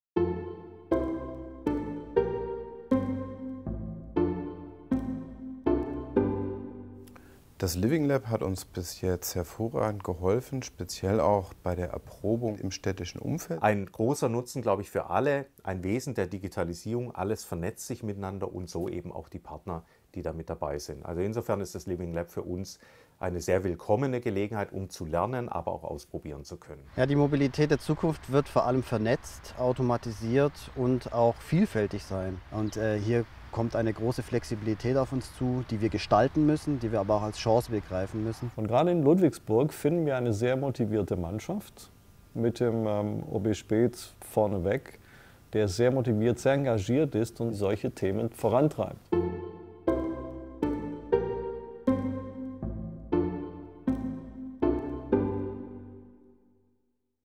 Living LaB Ludwigsburg - Das Innovations-Netzwerk stellt sich vor
Ganz unterschiedliche Projekte - ein Ziel: Wie gestalten wir den Prozess der Digitalisierung in allen Lebensbereichen, im Alltag von uns Menschen? (Filmproduktion: Florian Kläger; Idee und Redaktion: Jolanta Gatzanis) Mehr zum Ludwigsburger Innovations-Netzwerk aus Wirtschaft, Wissenschaft und Verwaltung